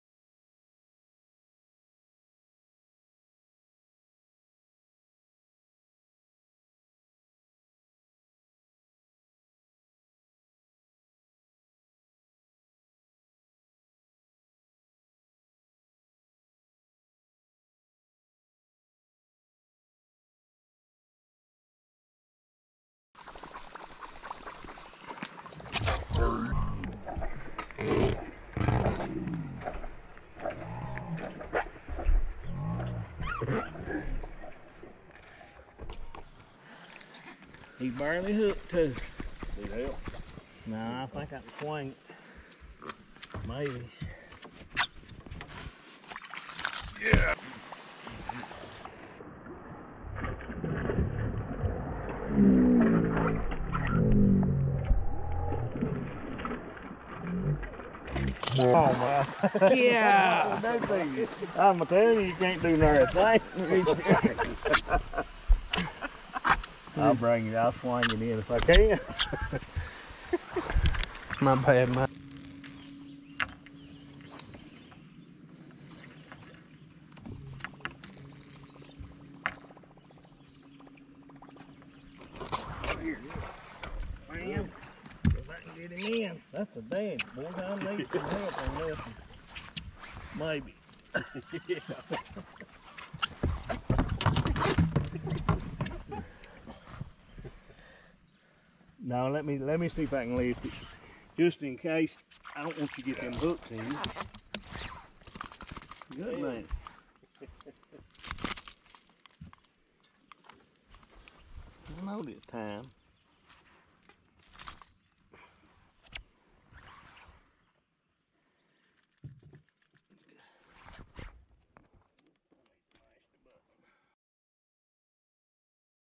Top water fishing Norris Lake
Top water bait was on until the rain set in